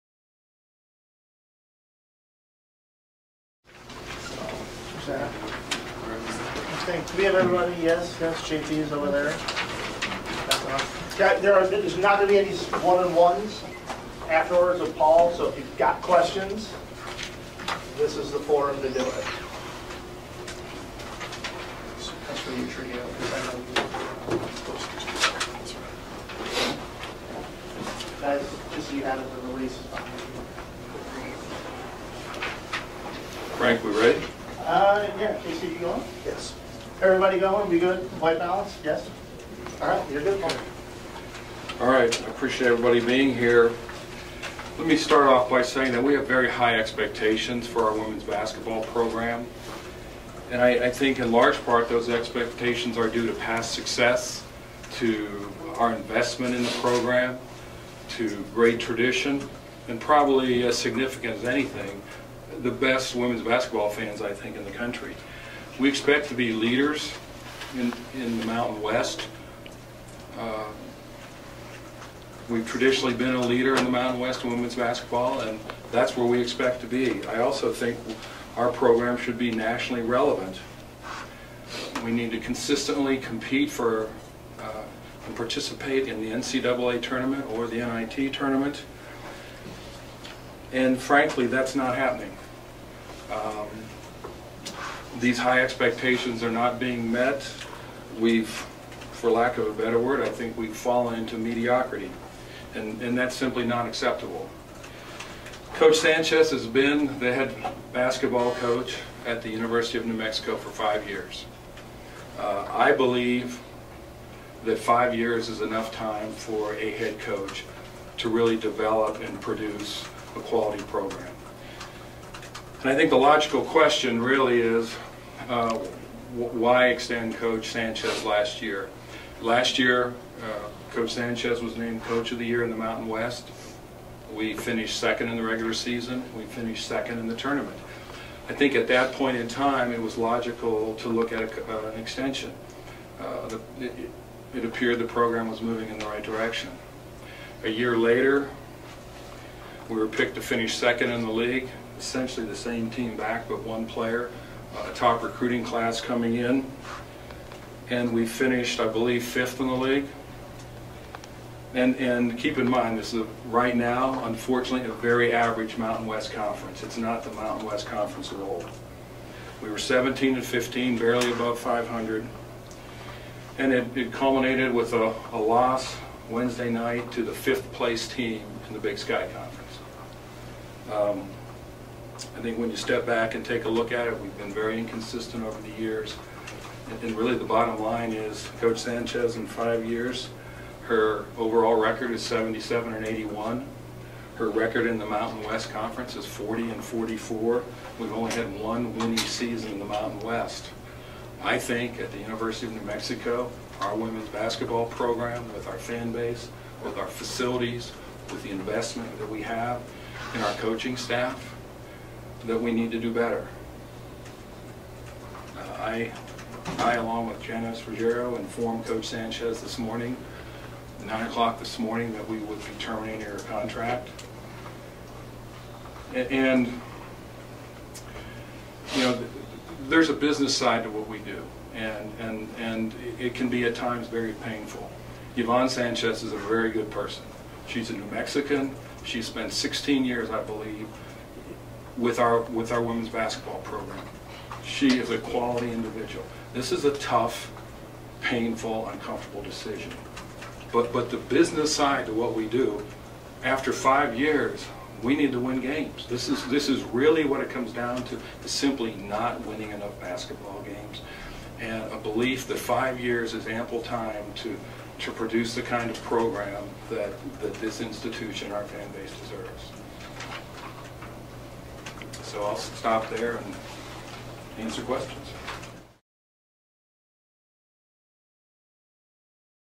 Yvonne Sanchez fired as UNM women's basketball coach
UNM's Paul Krebs holds press conference to talk about the firing of women's basketball coach Yvonne Sanchez. Video by Roberto E. Rosales / Journal

Krebs said the decision was performance-based. The Lobos went 77-81 under Sanchez and finished 17-15 this season, when they went 9-9 in Mountain West play and tied for fifth place.
New Mexico’s season ended Wednesday with a first-round home loss to Weber State in the Women’s Basketball Invitational.

“We have very high expectations for our women’s basketball program,” Krebs said. “… We expect to be leaders in the Mountain West and our program should be nationally relevant. We need to consistently participate in the NCAA Tournament or the (W)NIT and frankly, that’s not happening. For lack of a better word, we’ve fallen into mediocrity. That’s not acceptable.”